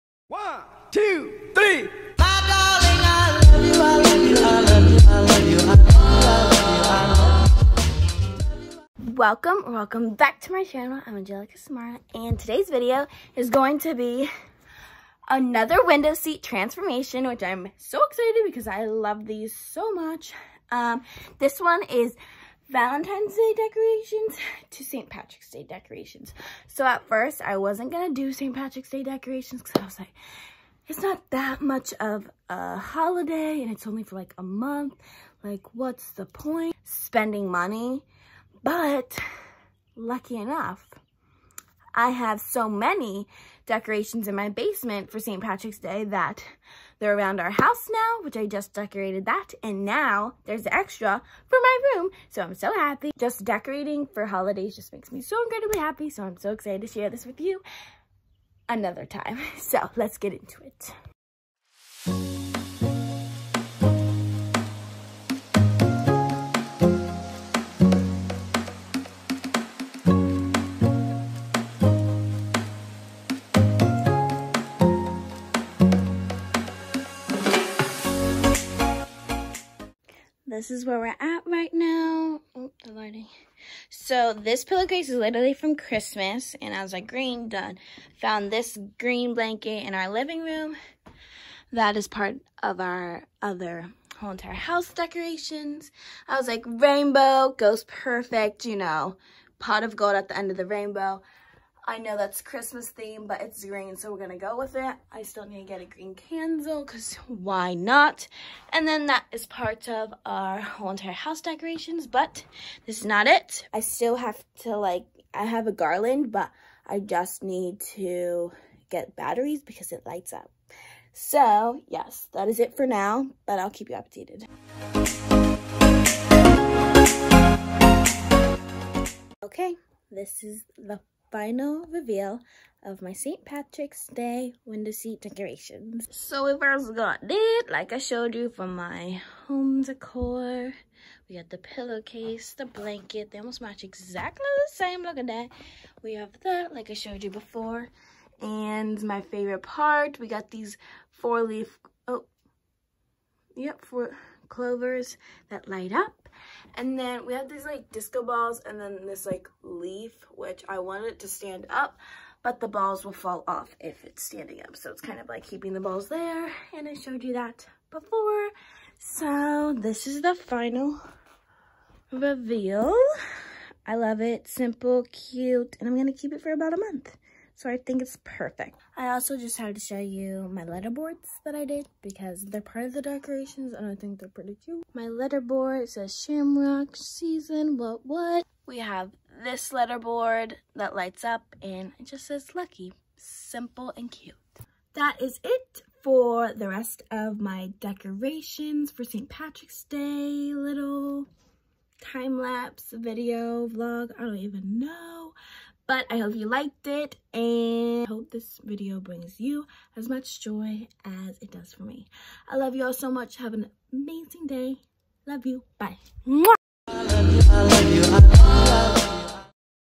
Decorating my window seat part 4
Hellooooo! Just another decorating my window seat video for saint Patrick’s day! Anyways I hope you like it! Love youuuu

All social media’s: @angelica_samarra